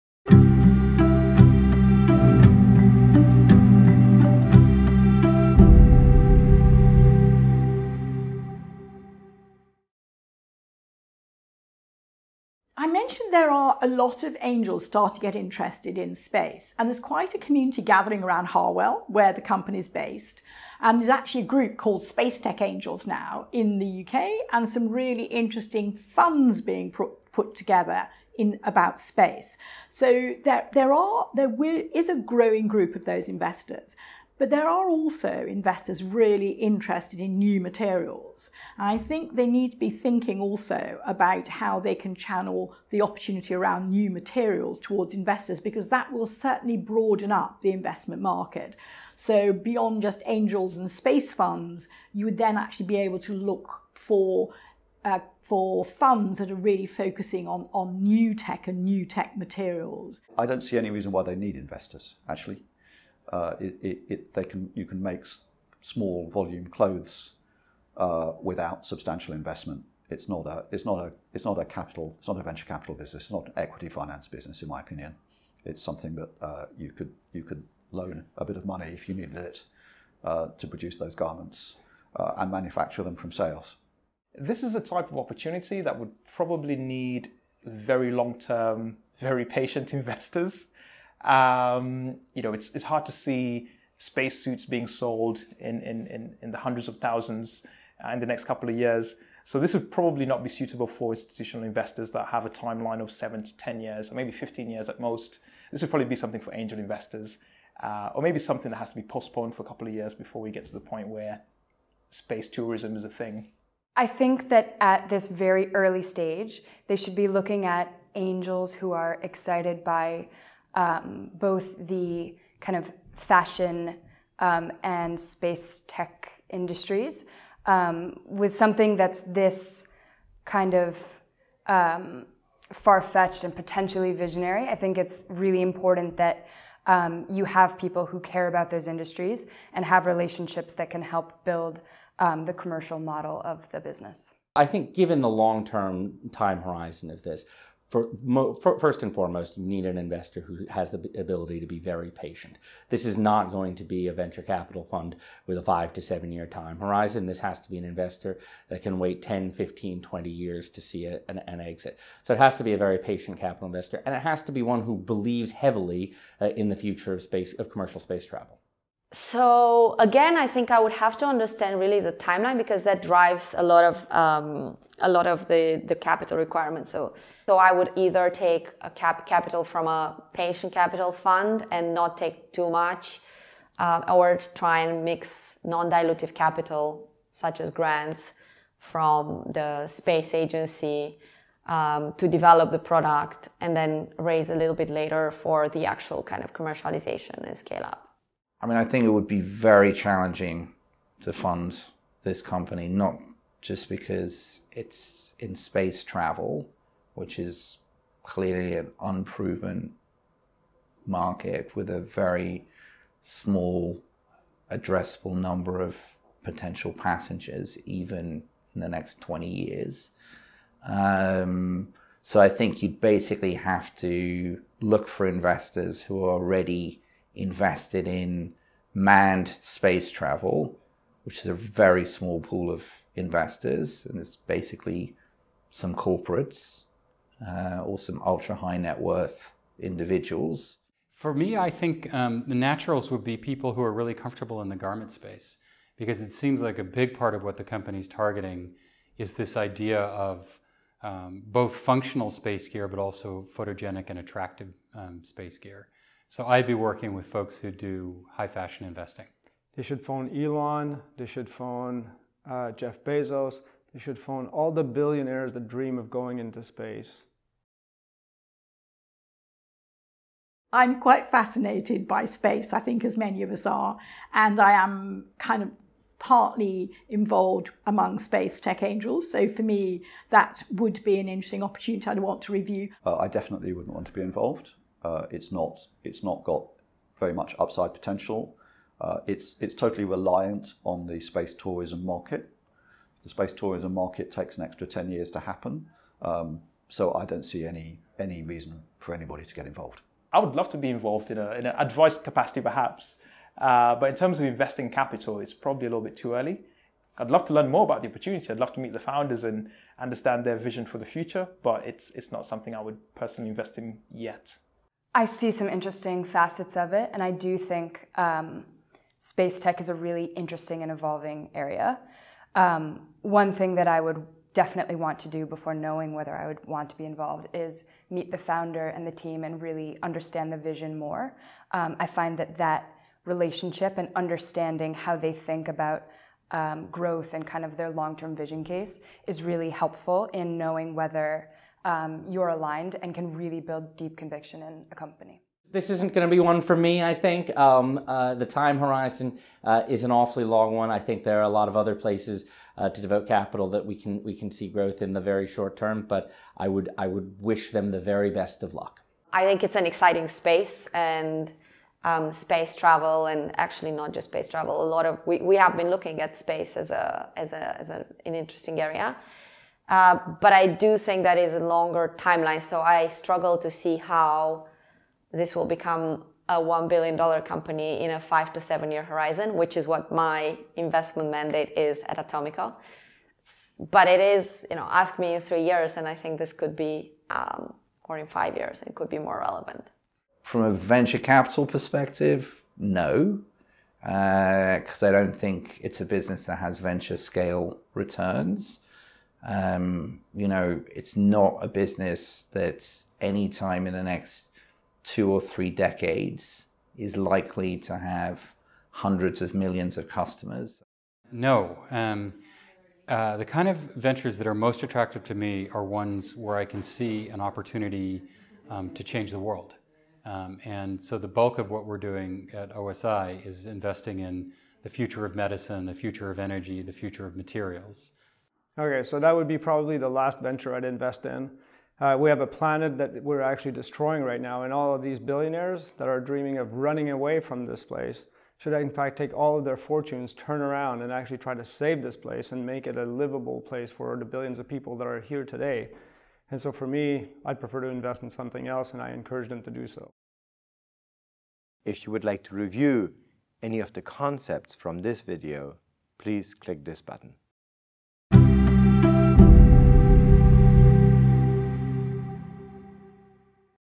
OXF ENT M3U1 Video Set 2 Video 3 | Industry experts Part 2
Industry experts respond to Tessilo, Ltd. Pitch; Part 2

0:00 Saïd Business School, Oxford Opening Credits
0:09 Jenny Tooth Perspective
1:03 William Tunstall-Pedoe Perspective
1:24 Michael Tefula Perspective
1:54 Charlotte Clune Perspective
2:26 Jeff Lynn Perspective
2:50 Irina Haivas Perspective
3:19 Saul Klein Perspective
4:02 Charles Conn Perspective
4:21 Patrick Pichette Perspective
4:29 Jenny Tooth Perspective
4:43 William Tunstall-Pedoe Perspective
5:01 Michael Tefula Perspective
5:15 Charlotte Clune Perspective
5:46 Jeff Lynn Perspective
6:00 Irina Haivas Perspective
6:30 Saul Klein Perspective
6:52 Charles Conn Perspective
7:11 Patrick Pichette Perspective Online Resources For Background Reading: